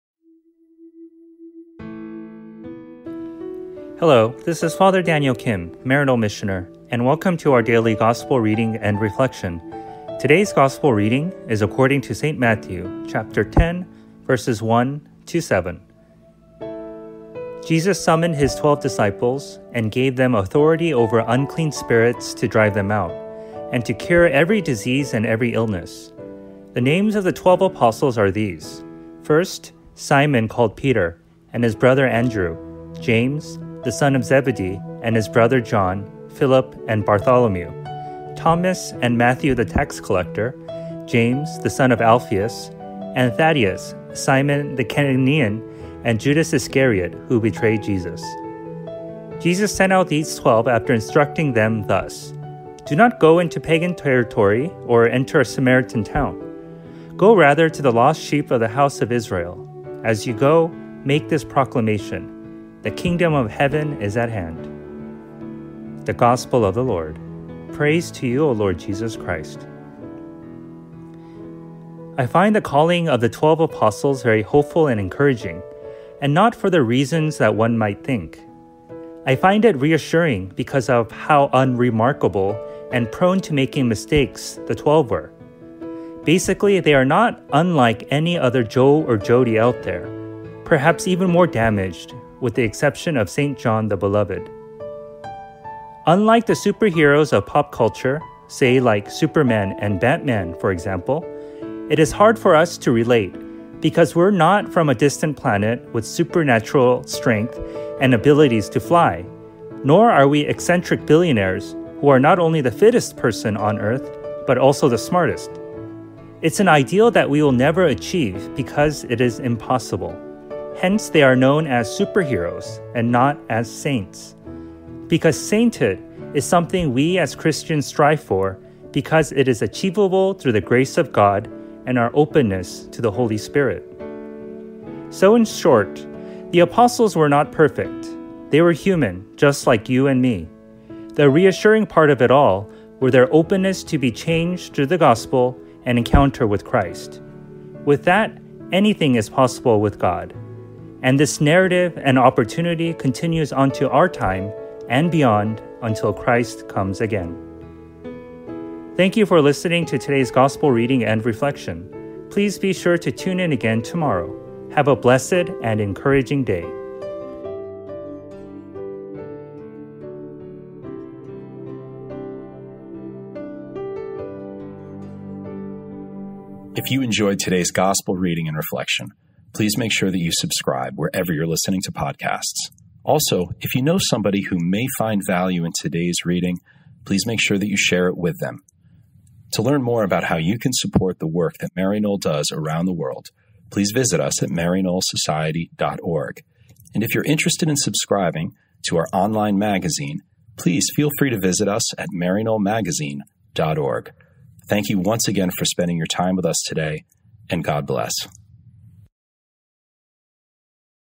Matthew 10:1-7, Daily Gospel Reading and Reflection | Maryknoll Fathers and Brothers
Join Fr. Daniel Kim, M.M. on the Wednesday of the Fourteenth Week in Ordinary Time
 as he reflects on:  Mt 10:1-7

As you go, make this proclamation: "The Kingdom of heaven is at hand."